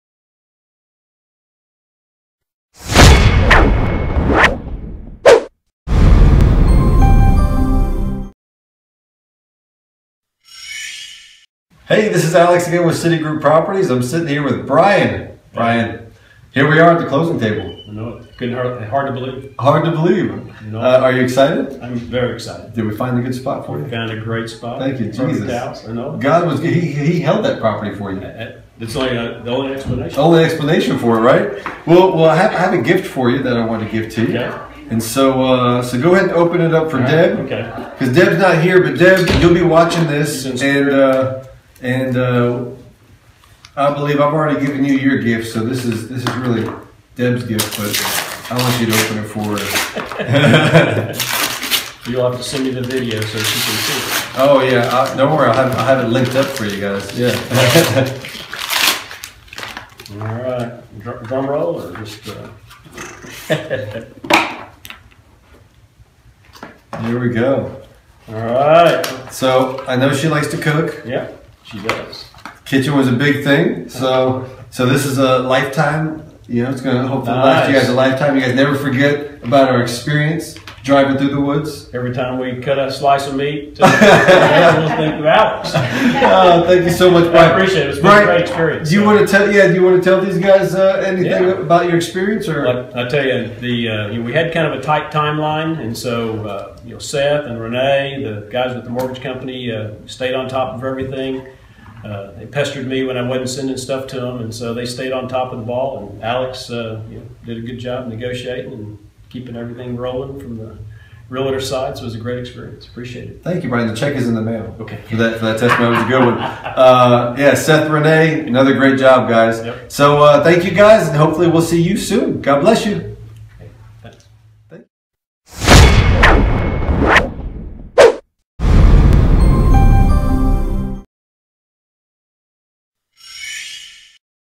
Alex DiSaggio - Brian Testimony - Closed Early on $750,000 Lake House
Brian and Deb got an amazing house on Lake Conroe - just what they were looking for, couldn't have been more perfect!!  And... we got the loan closed early on a $750,000 house!

Alex DiSaggio | City Group Properties

713-36-HOUSE